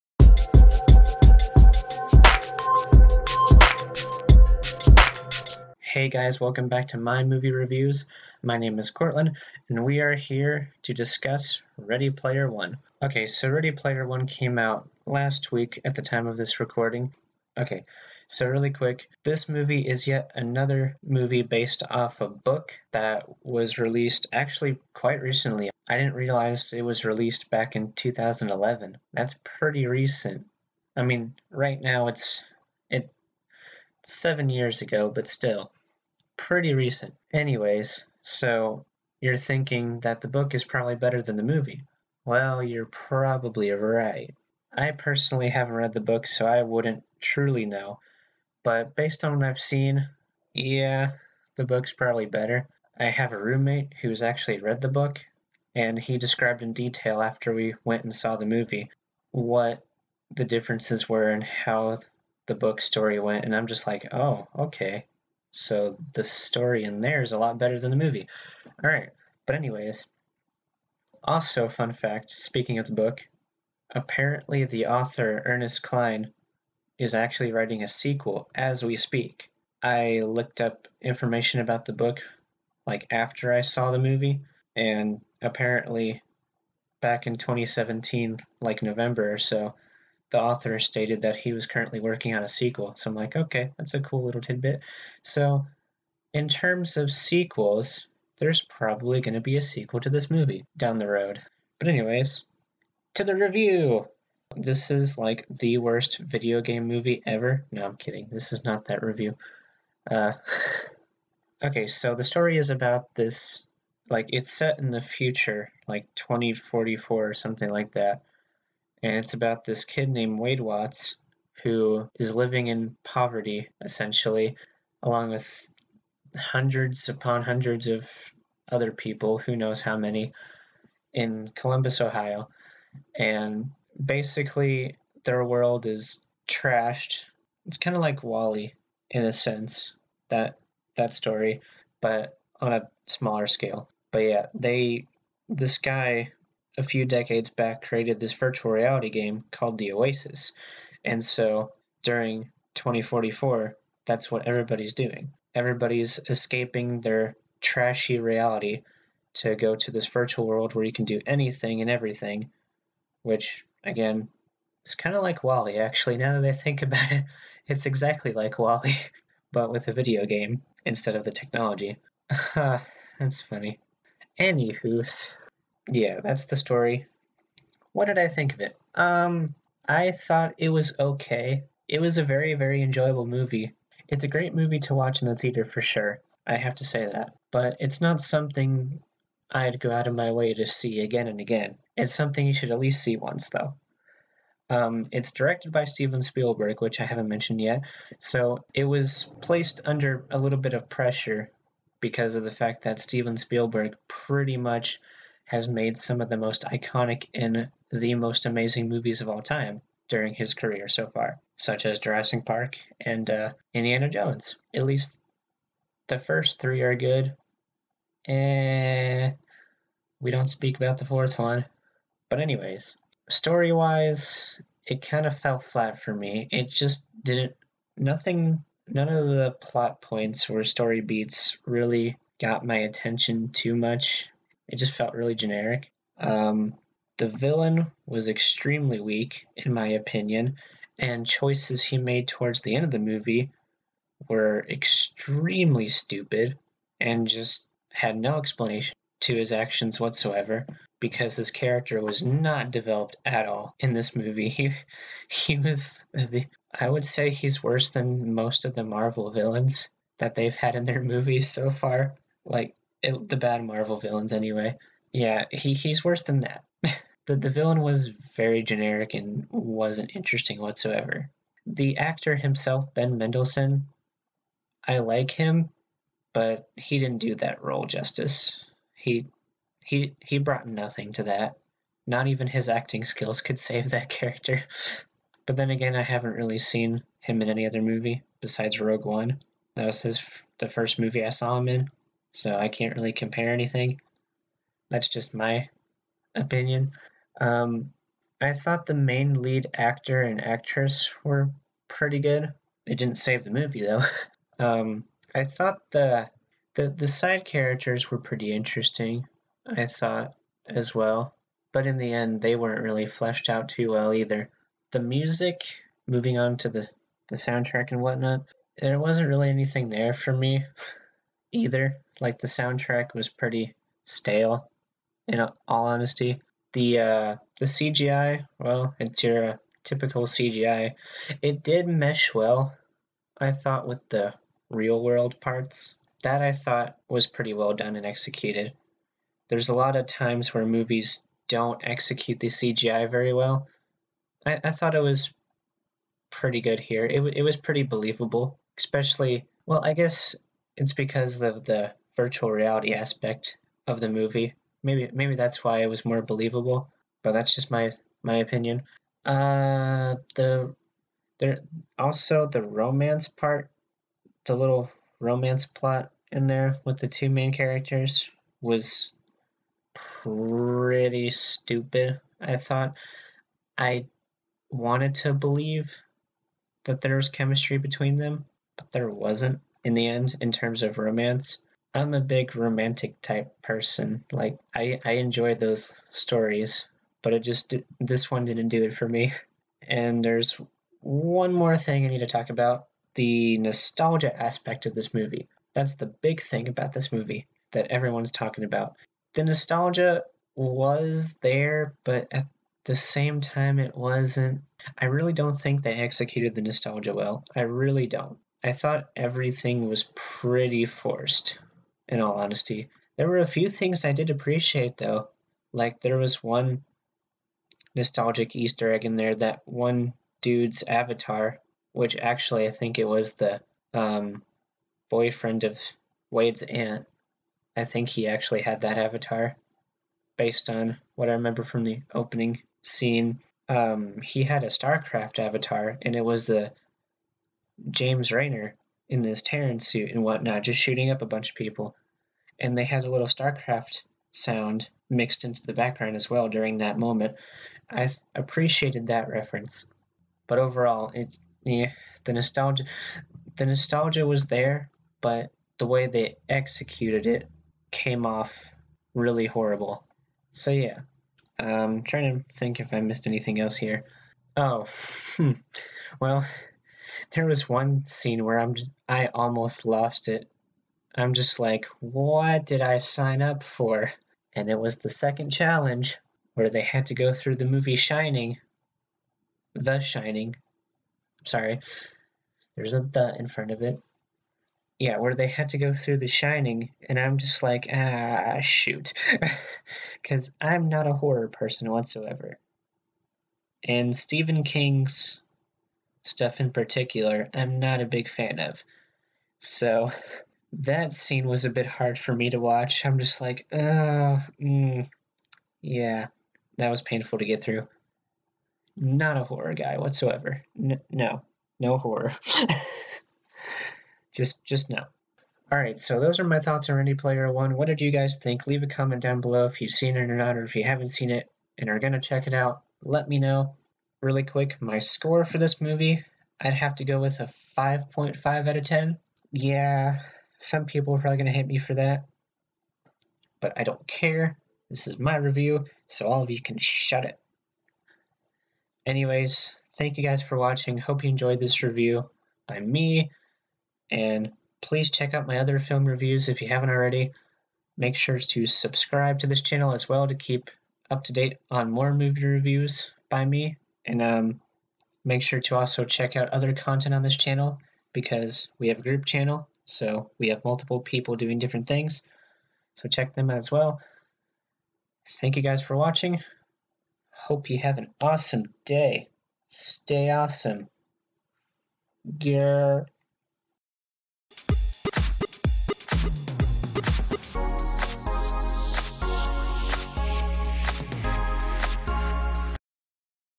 My Movie Reviews: Ready Player One
Does Ready Player One Hold up as a Great Spielberg masterpiece? Tune into the review as I share my thoughts on the movie and more

Synopsis: When the creator of a virtual reality world called the OASIS dies, he releases a video in which he challenges all OASIS users to find his Easter Egg, which will give the finder his fortune.

Comment at the bottom to give us your feed back.